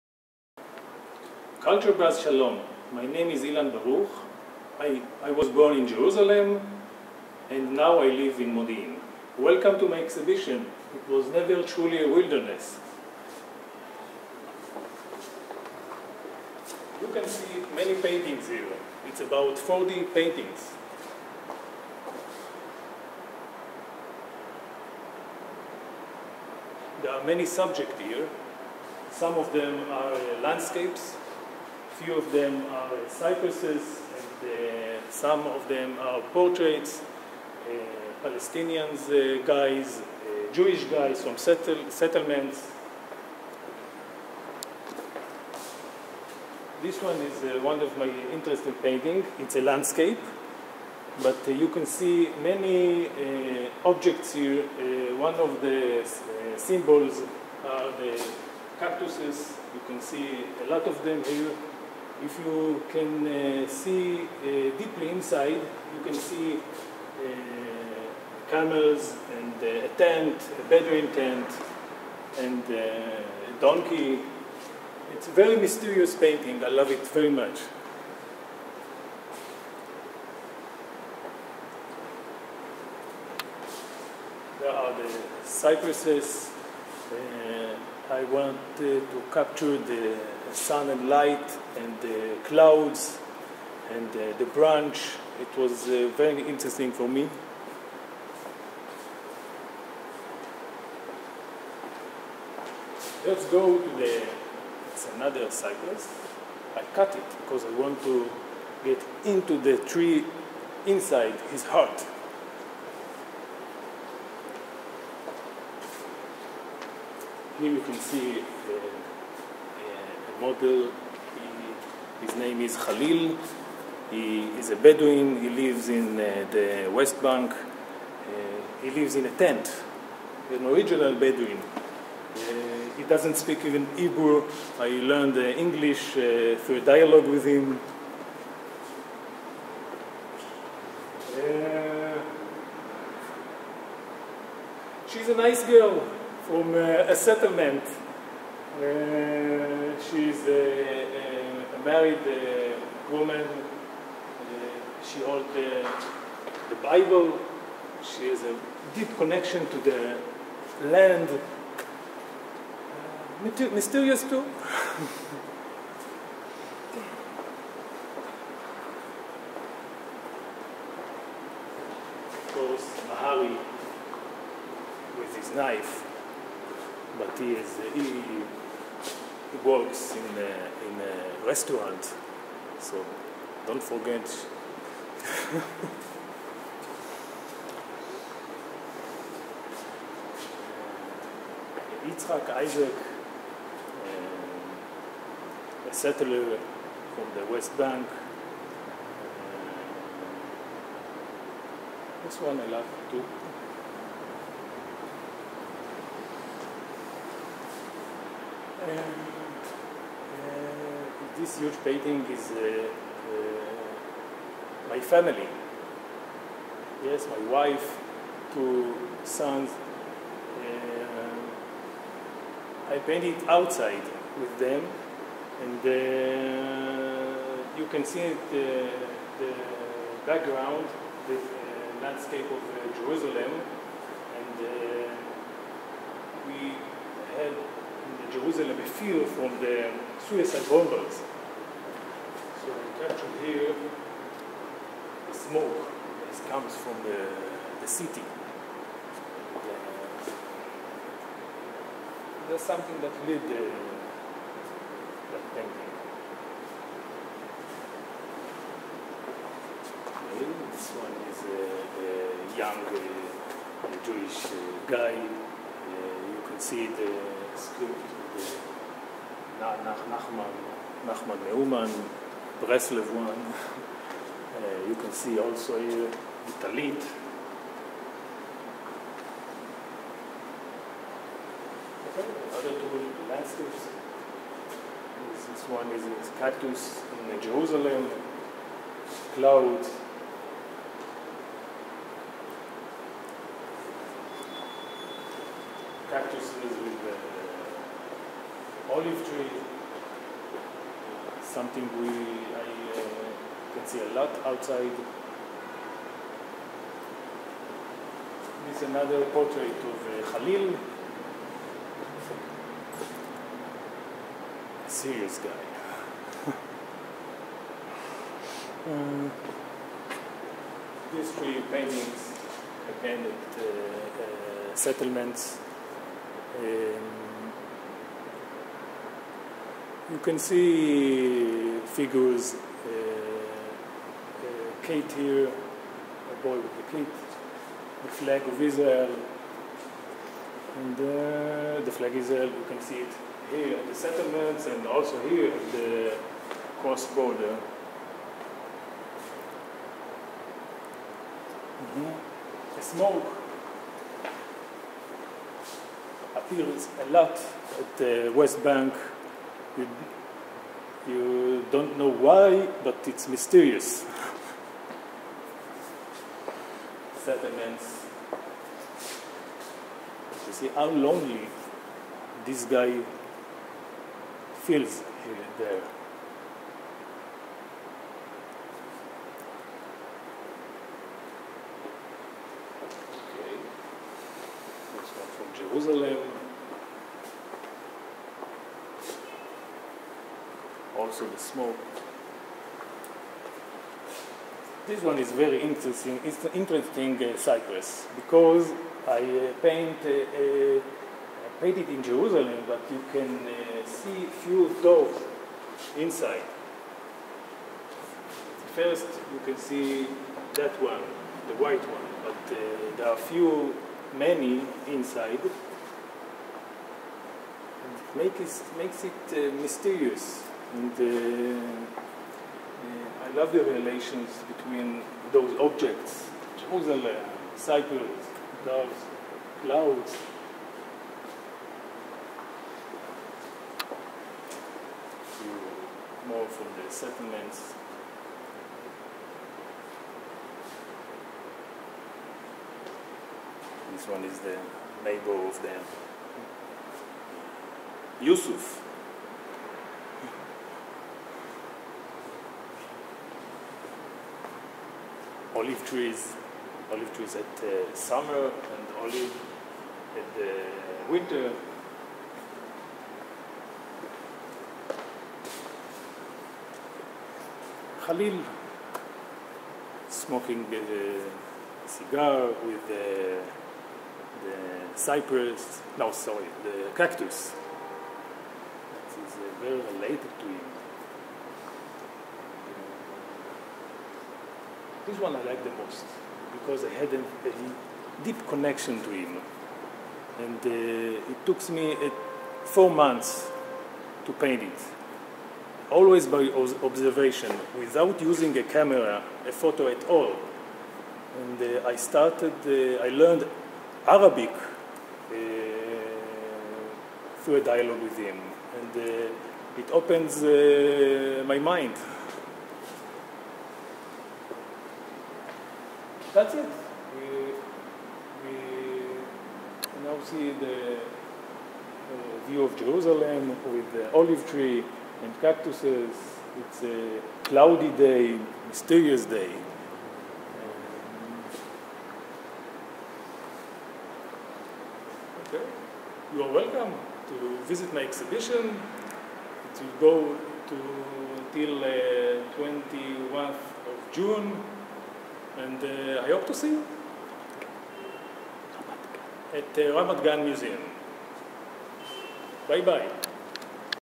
CultureBuzz's Ultimate Exhibition Guided Tour: Ilan Baruch & "It Was Never Truly A Wilderness"
Our 22nd chapter of a fascinating Series, providing you with the Ultimate Exhibition Guided Tours by either the artist, the curator, the Gallery owner or Museum director (& sometimes more than one)!

What: "It Was Never Truly A Wilderness".
Where: Ramat Gan Museum For Israeli Art, 146 Aba Hillel St., Ramat Gan.
Curator: Meir Aharonson.
When: 21 March - 25 May 2013.

Meet your guide: Ilan Baruch (The artist).